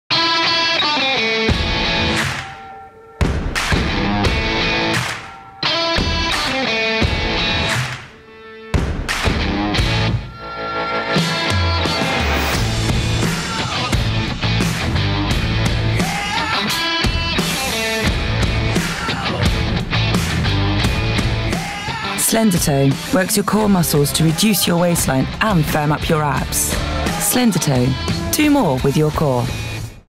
Work Your Core with Slendertone
Slendertone can help you look and feel amazing, improve strength and mobility, push further, jump higher, sit straighter and stand taller, but don’t just take our word for it. Find out how real people used our abs belts to improve the way they look, feel and move in their everyday lives